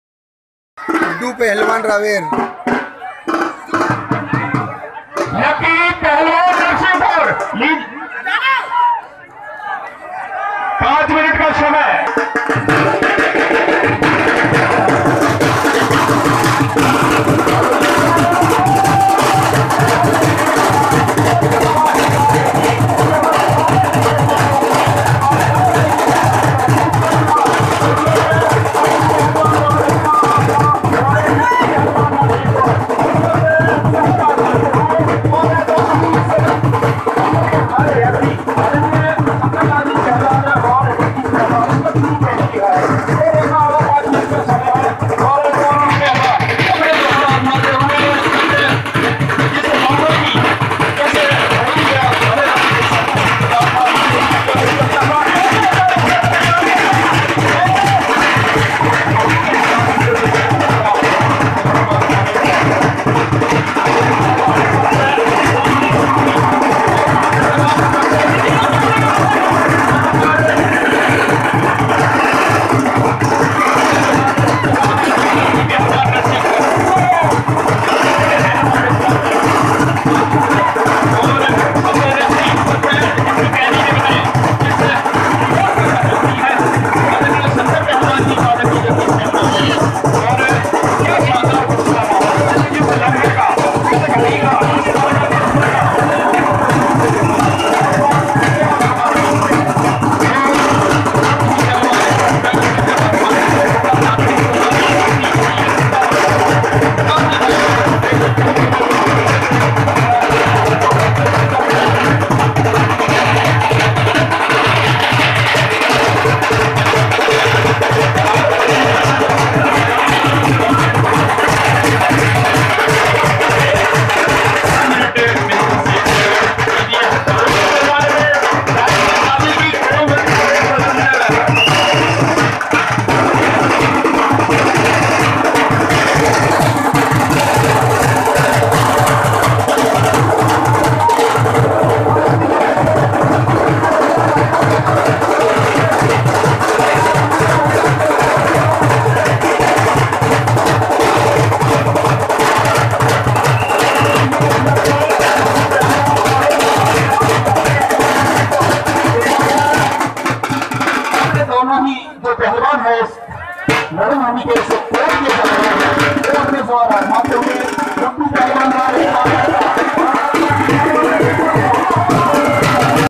गुड्डू पहलवान रावेर vs राजू पहलवान बनखेड़ी कुश्ती दंगल मध्य प्रदेश#altaf
श्री जय बजरंग व्यामशाला बनखेड़ी के तत्वधान में अखिल भारतीय विशाल इनामी दंगल संरक्षण पं सतपाल पलिया पूर्व अंतरराष्ट्रीय खिलाड़ी वेदांत माहेश्वरी रामदेव शुगर मिल बनखेड़ी सुमित पहलवान समस्त पहलवान  बनखेड़ी 10/3/2021